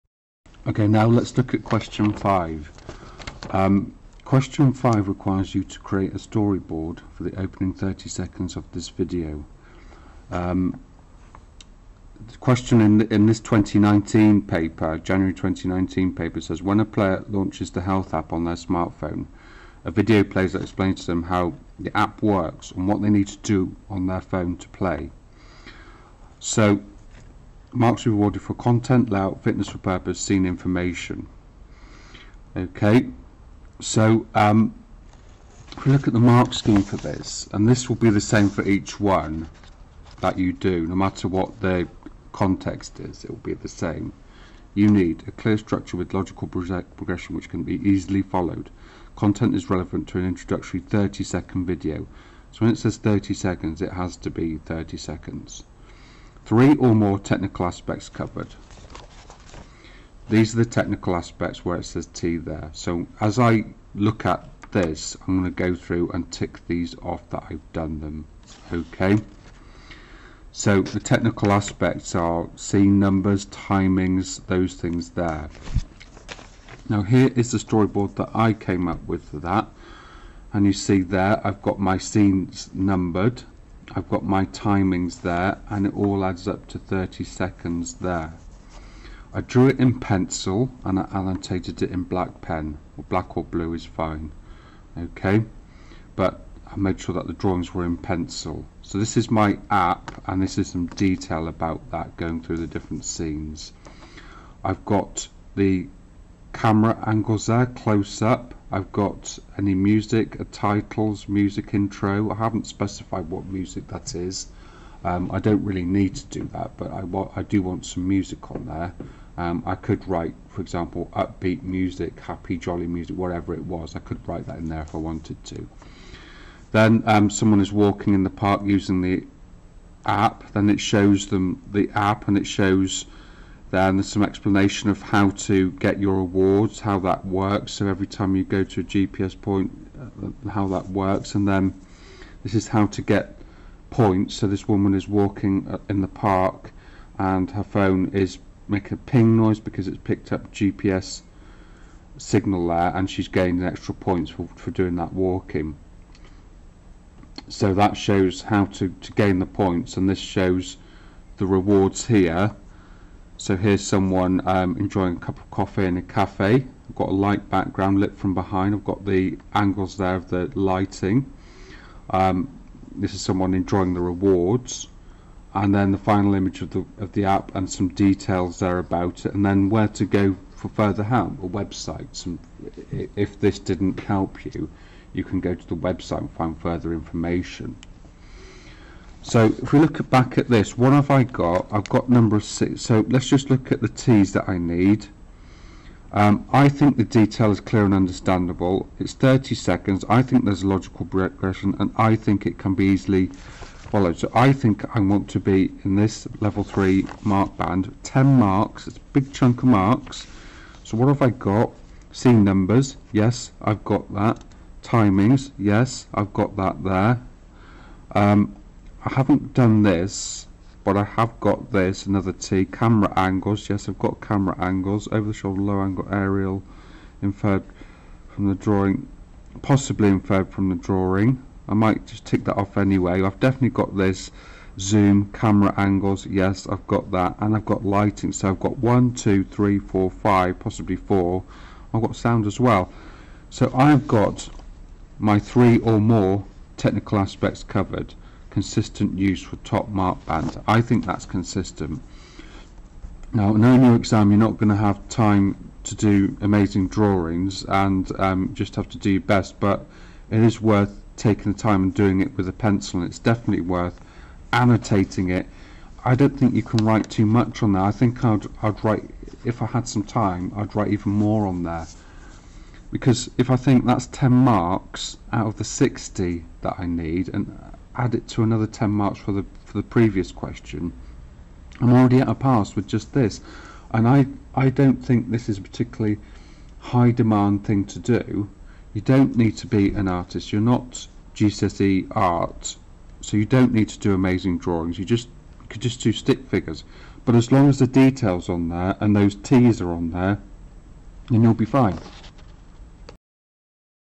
OCR iMedia Jan 2019 paper Question 5 Storyboard walk through
This is a walk through of question 5, the storyboard, of the OCR R081 iMedia paper.